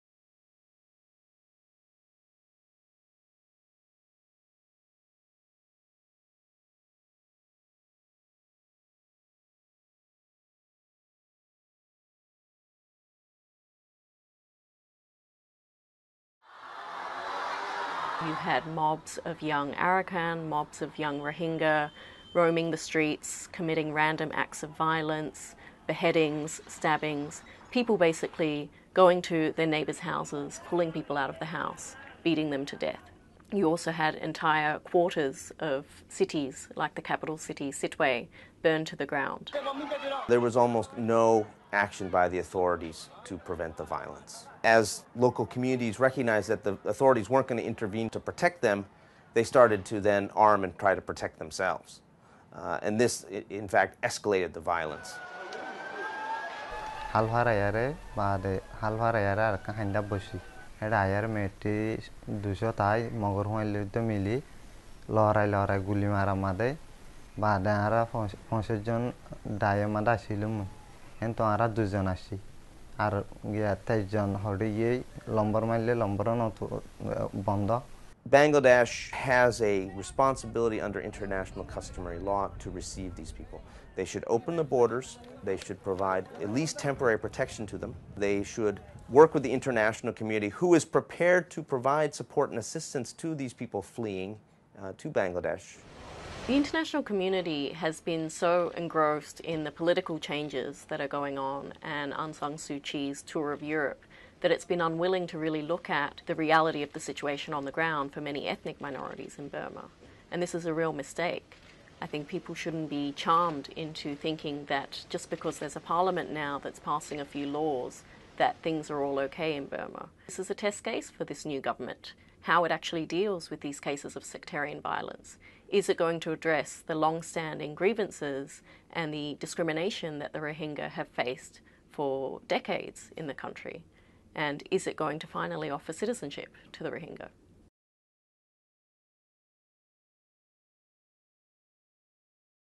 'Random Acts Of Violence'
A new Human Rights Watch report details atrocities during recent sectarian violence in Burma.